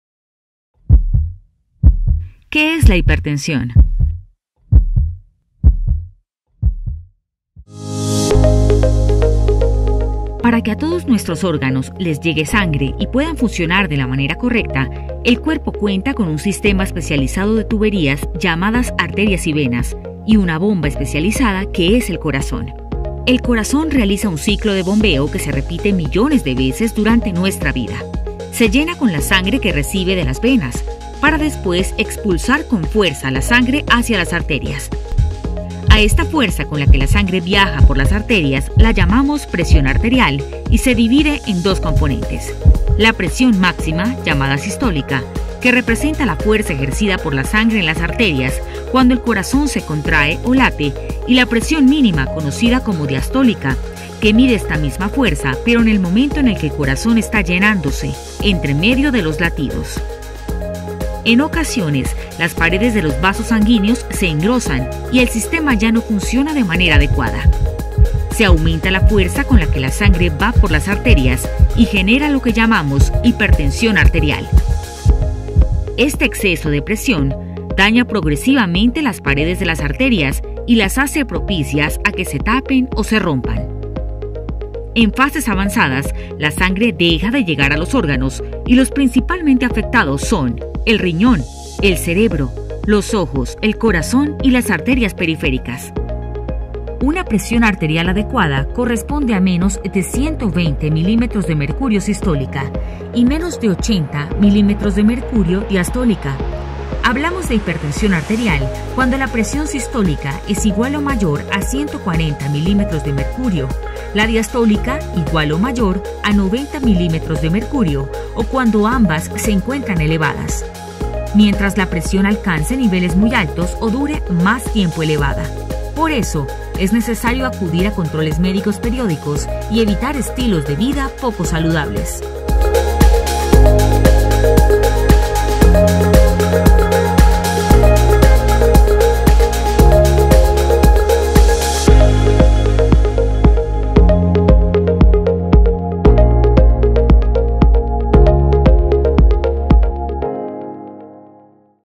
Día mundial de la hipertensión
DíaMundialDeLaHipertensión | La hipertensión es conocida como la ‘asesina silenciosa’ porque muchas veces no presenta sintomatología, nos unimos a la campaña del diagnóstico oportuno para disminuir la prevalencia de la enfermedad. Detalles